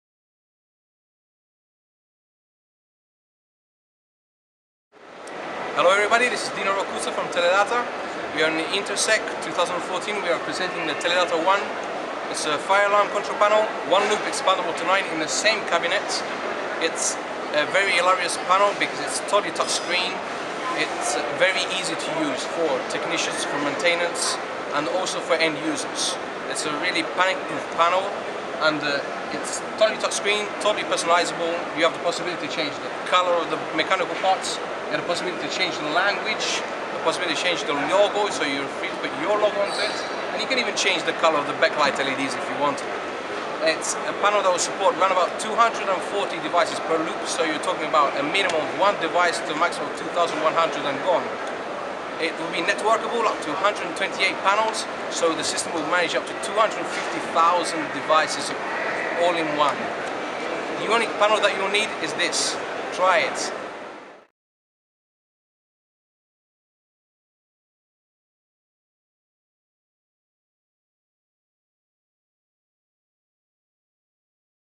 INTERSEC 2014 - Teledata Srl - Dino Andrea Roccuzzo (part 2)
TELEDATA ONE - Technical presentation by Dino Andrea Roccuzzo (Technical Expertise)